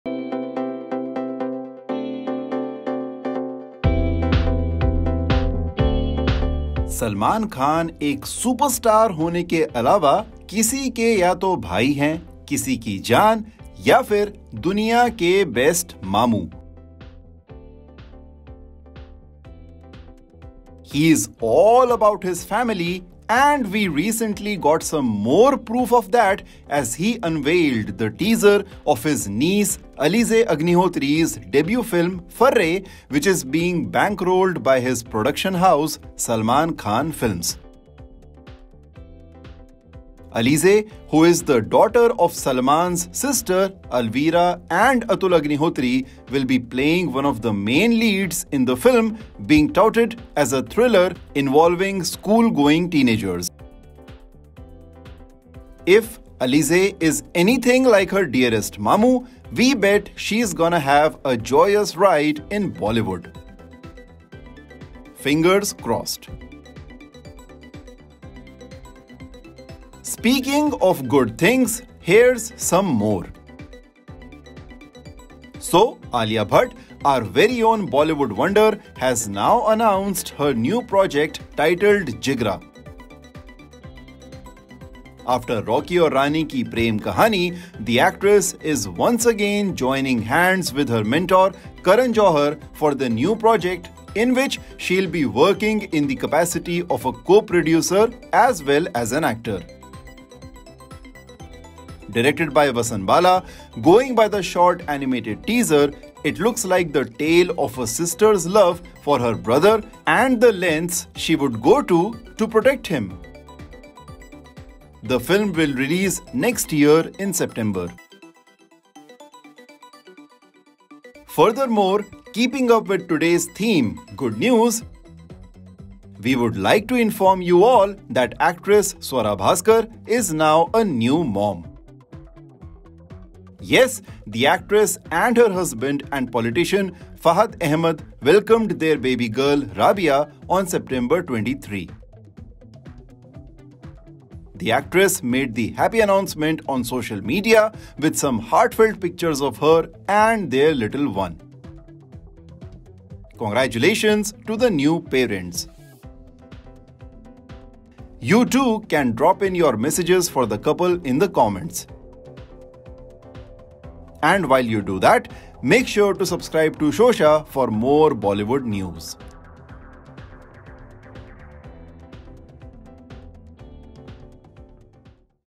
Salman Launches Niece Alizeh | Alia, KJo Reunite With Jigra | Swara Welcomes Baby Girl With Husband
Salman Khan has announced the debut film of his niece Alizeh Agnihotri. The film, titled 'Farrey' is being bankrolled by SK Films. In other news, Alia Bhatt has joined hands with her mentor Karan Johar for 'Jigra', a Vasan Bala film. Meanwhile, Swara Bhasker and her husband Fahad Ahmad are now parents. The couple welcomed their first born, a baby girl, on September 23. Watch today's entertainment news wrap for all the details.